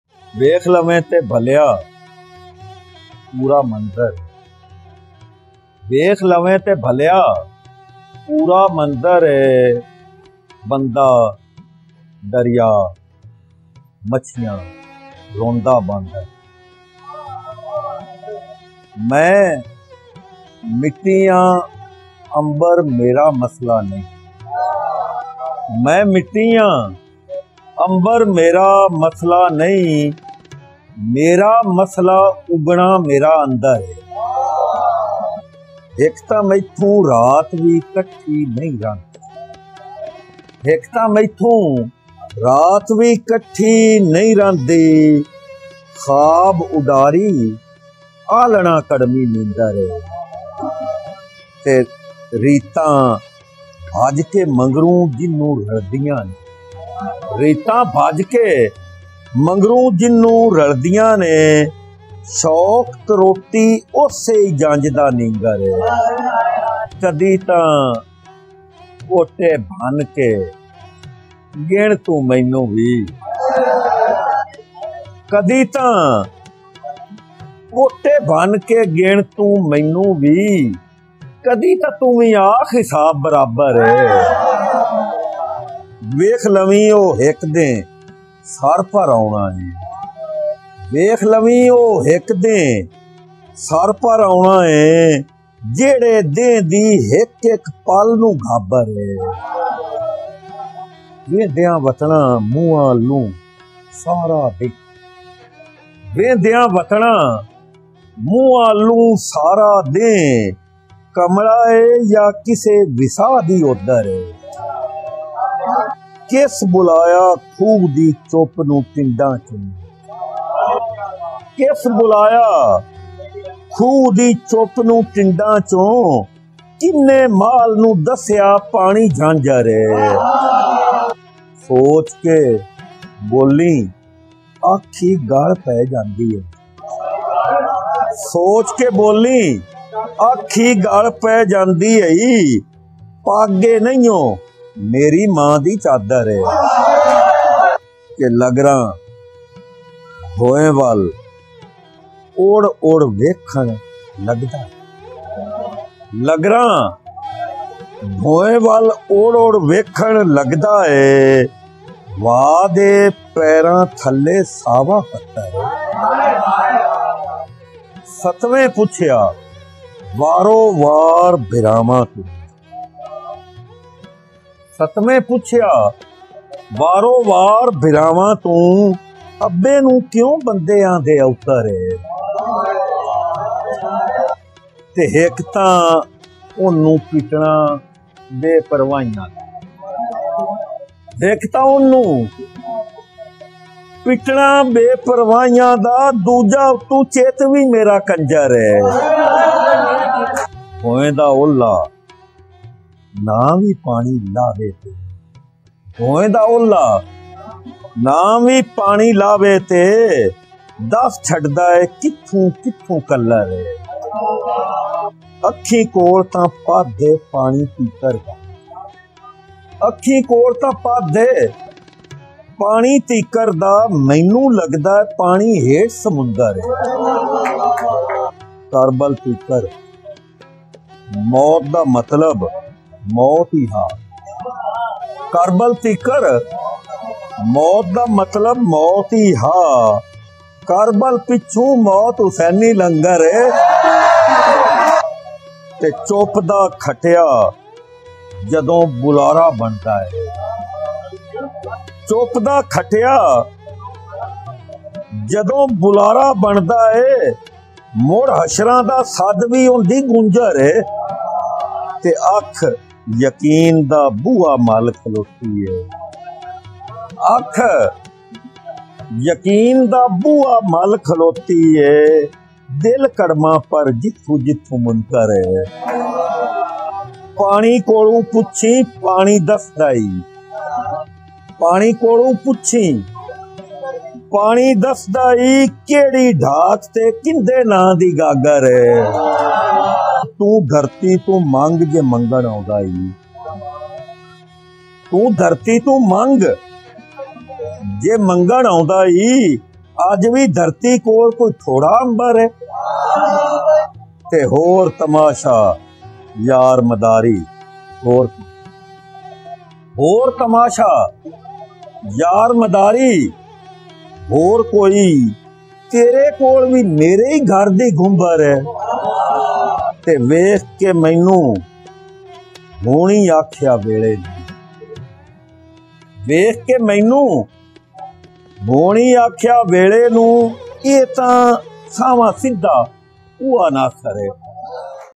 Wekh Lavien Tu Bhaleya Poora Manzar Ay Rai Muhammad Khan Nasir Sad Romantic Punjabi Poetry Ghazal
Wekh Lavein Tu Bhaleya Poora Manzar Ay  Latest Most Sad Romantic Love Top Trending Punjabi Poetry Whatsapp Status 2024
Poet: Rai Muhammad Khan Nasir

ویکھ لویں تے بھلیا پورا منظر اے
بندہ دریا مچھیاں روندا باندر اے

میں مٹی آں امبر میرا مسئلہ نئیں
میرا مسئلہ اگنا میرا اندر اے

ہک تاں میتھوں رات وی کٹھی نئیں رہندی
خواب اڈاری آہلنا کڑمی نیندر اے

ریتاں بھج کے مگروں جہنوں رلدیاں نیں
شوق تروٹی اوسے ای جنج دا نینگر اے

کدی تاں پوٹے بھن کے گن توں مینوں وی
کدی تاں توں وی آکھ حساب برابر اے

ویکھ لویں او ہک دینہہ سر پر آؤنا اے
جیہڑے دیہنہ دی ہک ہک پل نوں گھابر اے

ویہندیاں وتناں  مونہاں ولوں سارا دیہنہ
کملا اے یا کسے وساہ دی اودر اے

کس بلایا کھوہ دی چپ نوں ٹنڈاں چوں
کہنے مال نوں دسیا پانی جھانجر اے

سوچ کے بولیں آکھی گل پے جاندی ہئی
پگ اے نہینؤں میری ماں دی چادر اے

لگراں  بھوئیں ول اُڑ اُڑ ویکھن لگدا اے
واء دے پیراں تھلے ساوا پتر اے

ستویں پچھیا وار و وار بھراواں توں
ابے نوں کیوں بندے آہندے اوتر اے

ہک تاں اوہنوں پٹناں بے پروائیاں دا
دوجا اتوں چِت وی میرا کنجر اے

بھوئیں دا اوہلا نہ وی پانی لاہوے تے
دس چھڈدا اے کتھوں کتھوں کلر اے

اکھیں کول تاں پندھ اے پانی تیکر دا
مینوں لگدا اے پانی ہیٹھ سمندر اے

کربل تیکر موت دا مطلب موت ای ہا
کربل پچھوں موت حسینی لنگر اے

چپ دا کٹھیا جدوں بلارا بندا اے
مُڑ حشراں دا سد وی اوہندی گونجر اے

اکھ یقین دا بوہا مل کھلوتی اے
دل کڑماں پر جتھوں جتھوں منکر اے

پانی کولوں پچھیں پانی دسدا ای
کیہڑی ڈھاک تے کہندےناں دی گاگر اے

توں دھرتی توں منگ جے منگن آندا ای
اج وی دھرتی کول کوئی تھوڑا امبر اے

ہور تماشہ یار مداری ہور کوئی
تیرے کول وی میرے ای گھر دی گھمبر اے

ویکھ کے مینوں ہونی آکھیا ویلے نوں
اے تاں ساہنواں سدھا اوہا ناصر اے

رائے محمد خاں ناصر